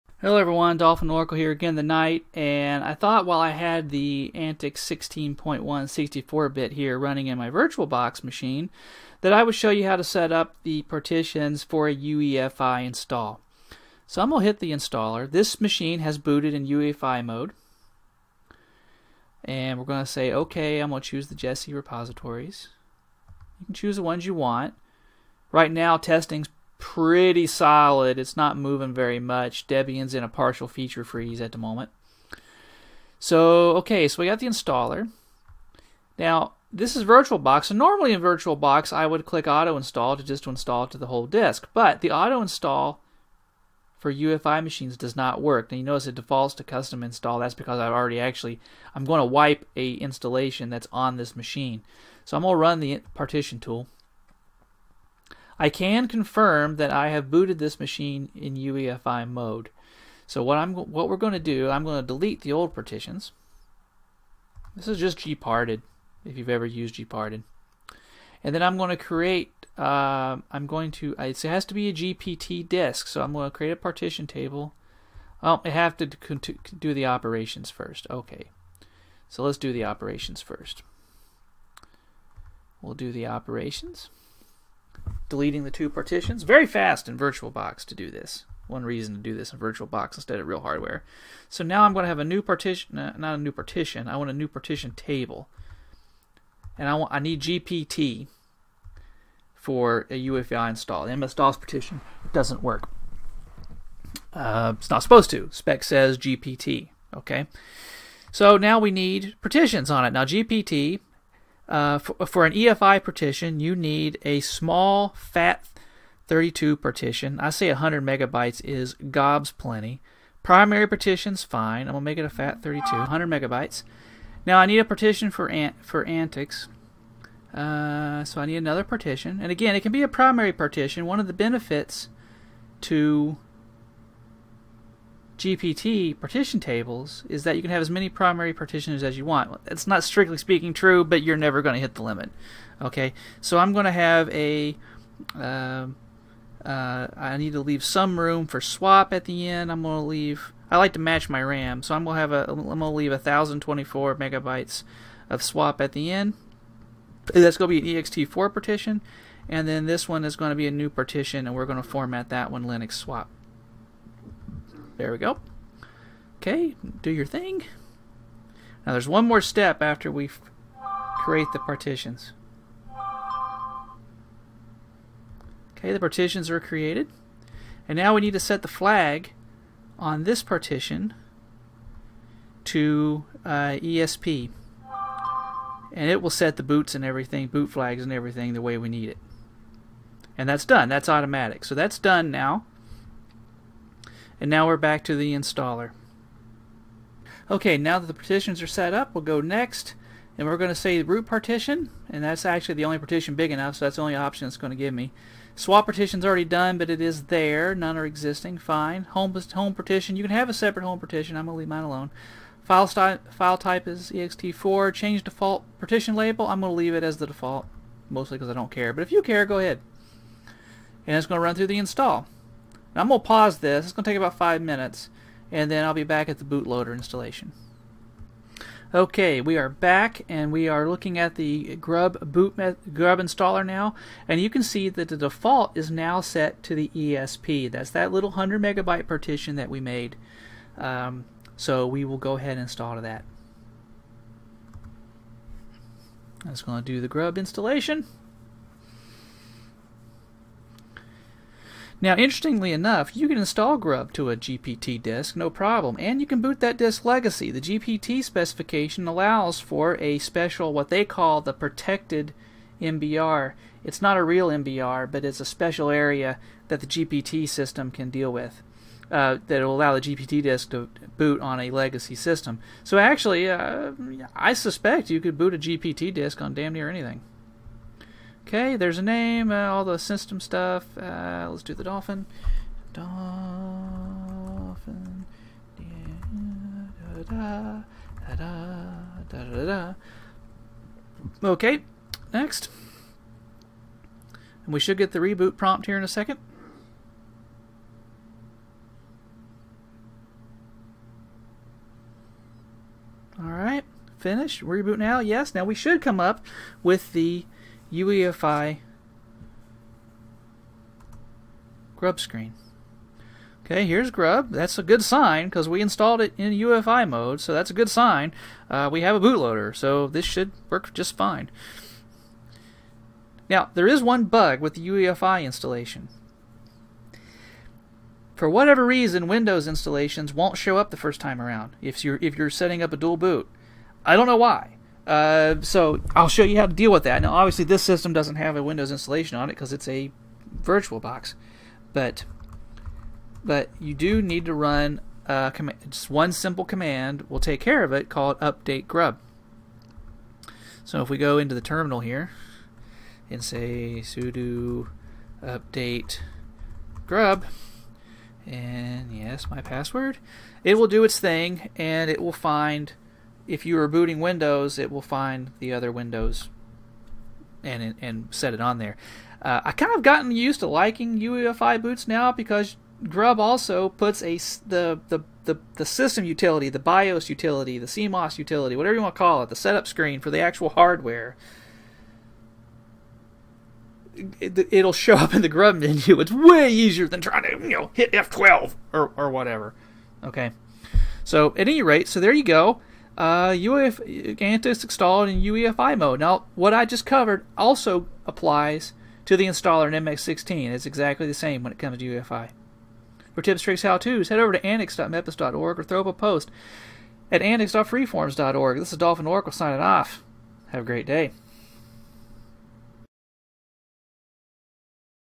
antiX 16 - UEFI install (with partitioning)
Install antiX 16.1 on UEFI hardware.  I also show off how to partition for UEFI on a clean single boot system.